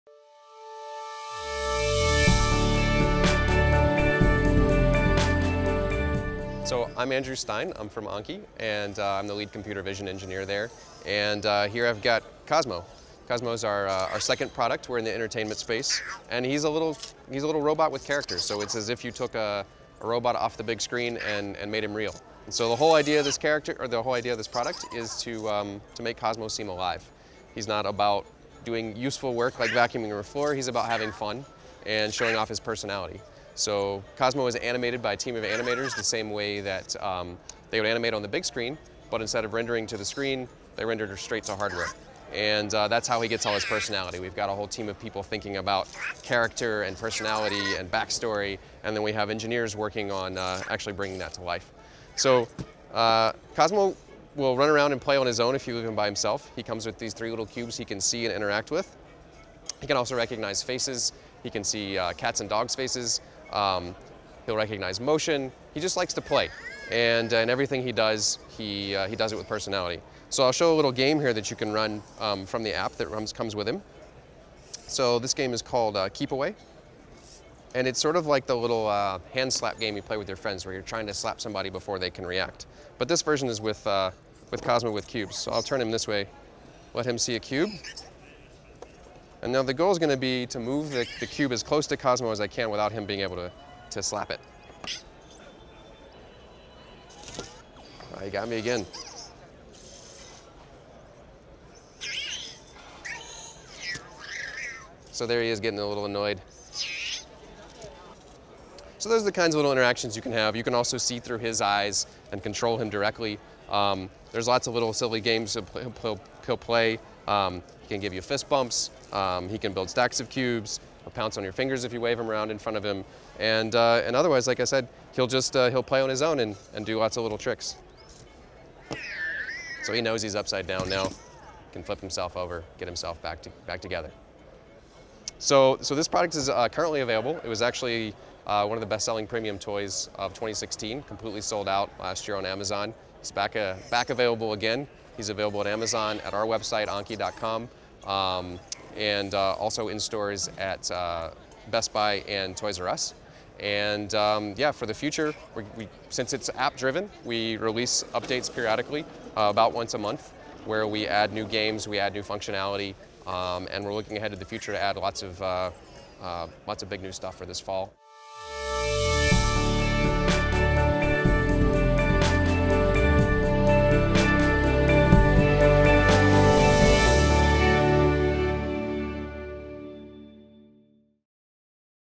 Anki Demonstration of the Cozmo Entertainment Robot
Andrew Stein, Lead Computer Vision Engineer at Anki, demonstrates the company's latest embedded vision technologies and products at the May 2017 Embedded Vision Summit. Specifically, Stein demonstrates Cozmo, a highly successful entertainment robot released by Anki in Fall of 2016. Stein discusses the use of personality in bringing Cozmo to life, talks about the robot's capabilities, and demonstrates one of the app-driven games he can play.